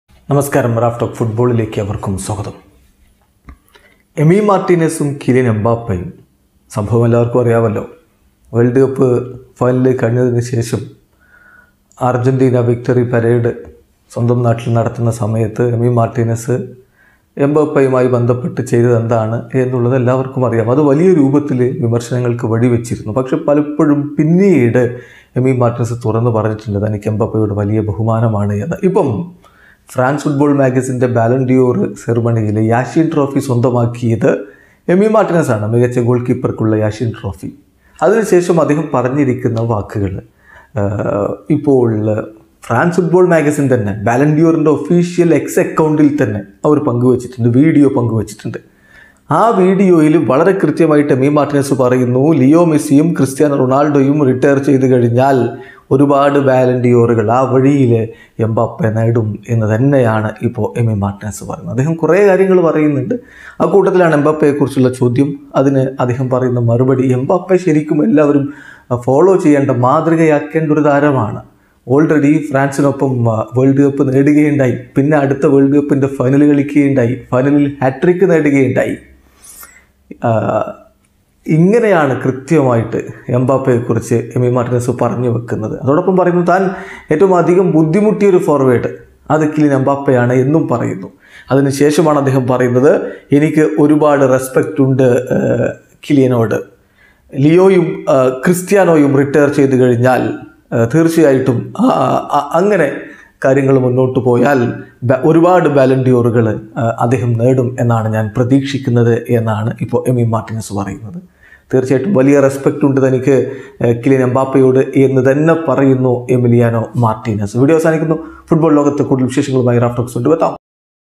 എംബപ്പേ ഒരുപാട് ബാലാൻഡിഓറുകൾ നേടും: എമി മാർട്ടിനെസ് | Emi Martinez | Kylian Mbappe
Yachine Trophy
Emiliano Martinez
Kylian Mbappe
Ballon d'Or 2023